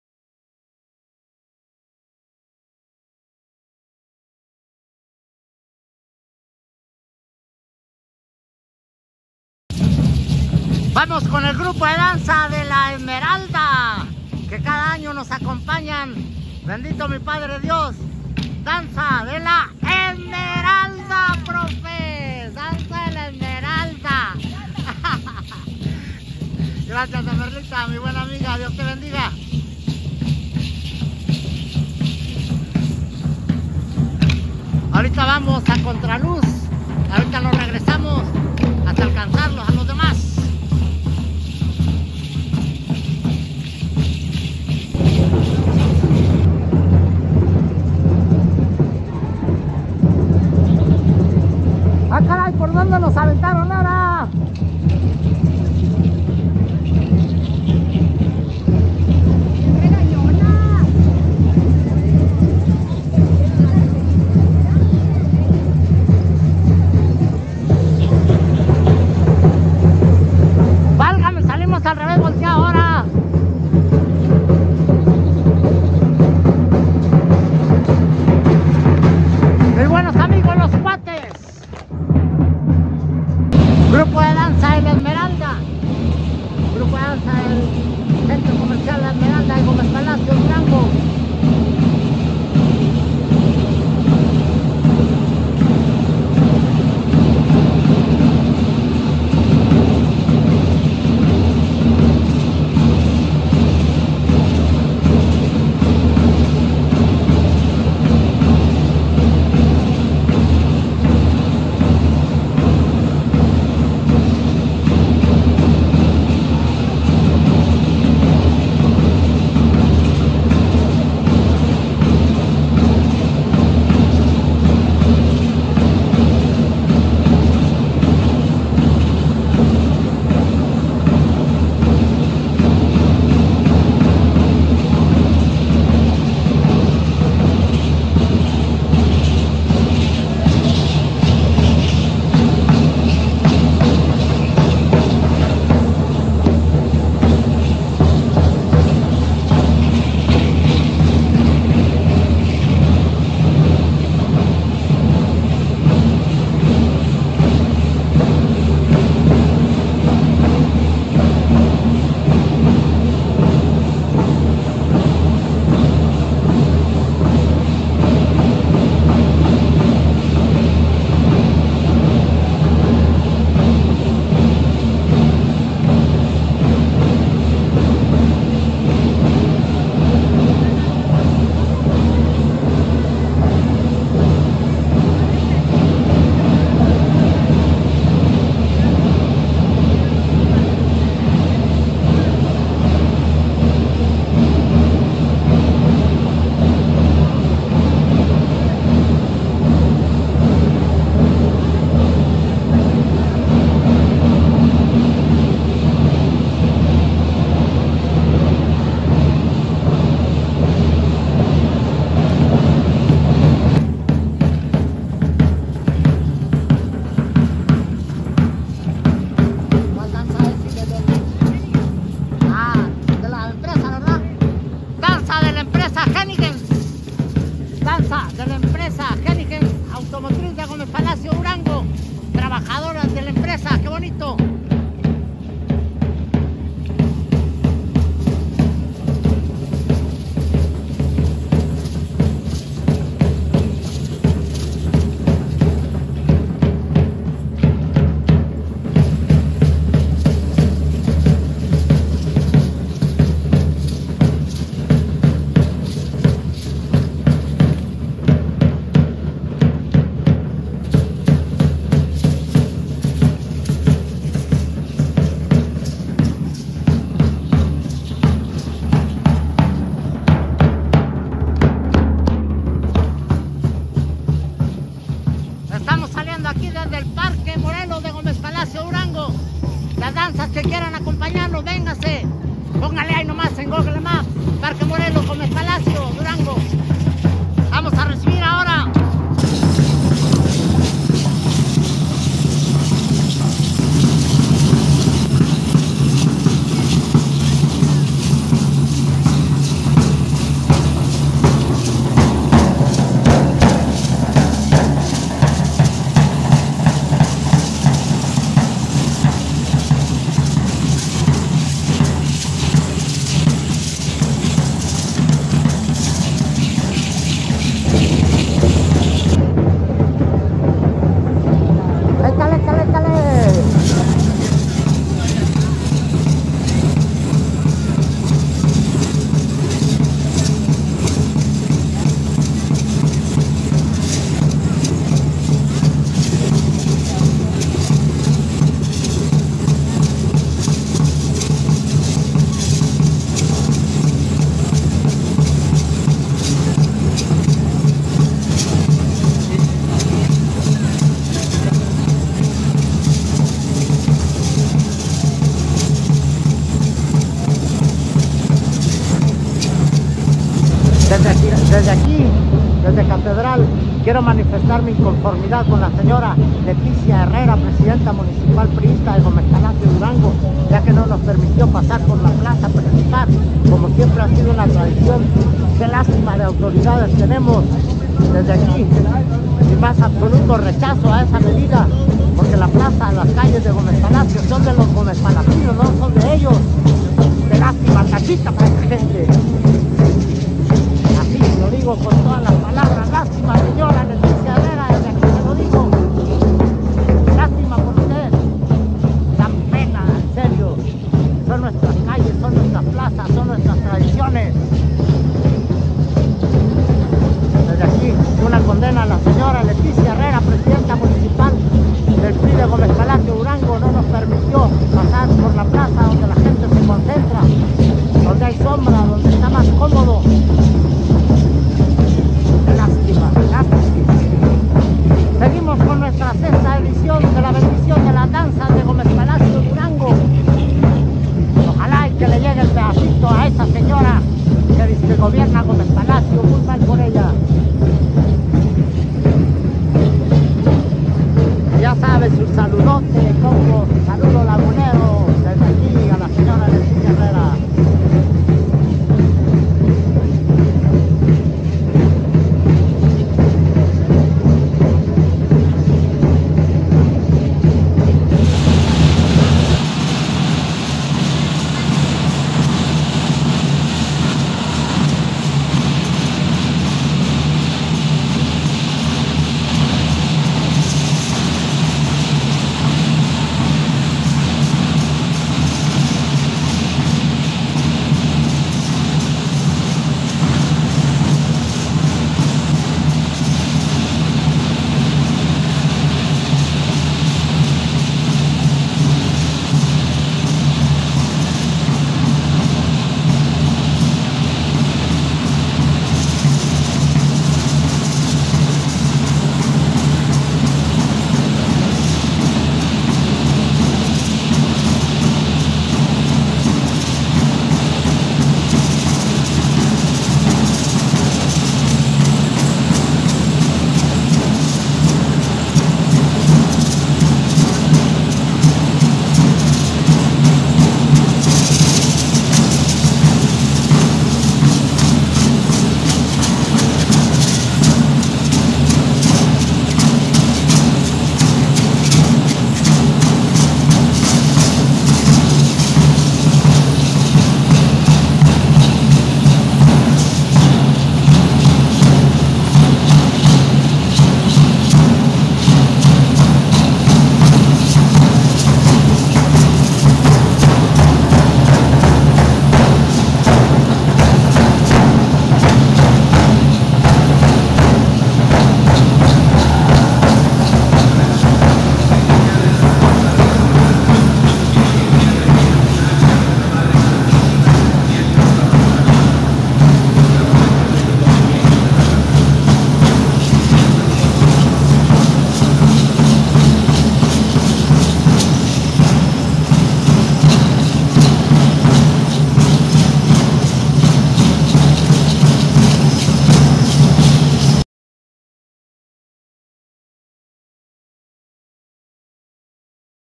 Danzas La Esmeralda Centro Comercial y Henniges Automotive BENDICIÓN DANZAS GÓMEZ PALACIO, DGO. 2023
BENDICIÓN DE LAS DANZAS GÓMEZ PALACIO, DGO. 2023. BLOQUE 1 a 26 de noviembre 2023.
*Danza "La Esmeralda" Centro Comercial La Esmeralda, Gómez Palacio, Dgo.
* Danza "Henniges Automotive" Empresa Automotriz, Gómez Palacio, Dgo.